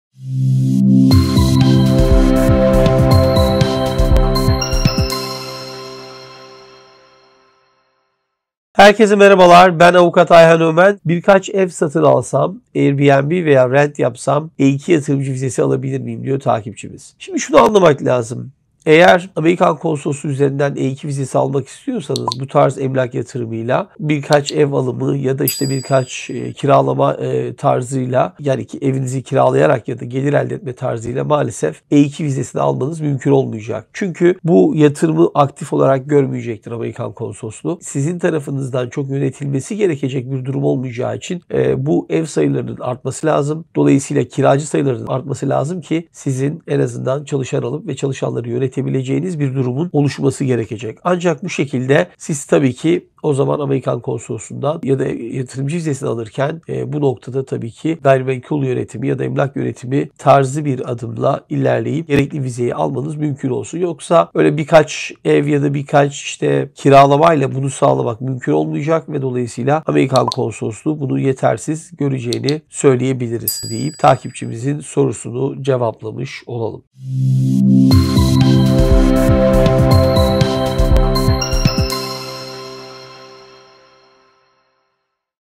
AMERİKA’DA EV ALIP AIRBNB’DE KİRAYA VERİP E2 YATIRIMCI VİZESİ ALABİLİR MİYİM ?
Soru: Birkaç ev satın alsam airbnb veya rent yapsam E2 yatırımcı vizesi alabilir miyim ?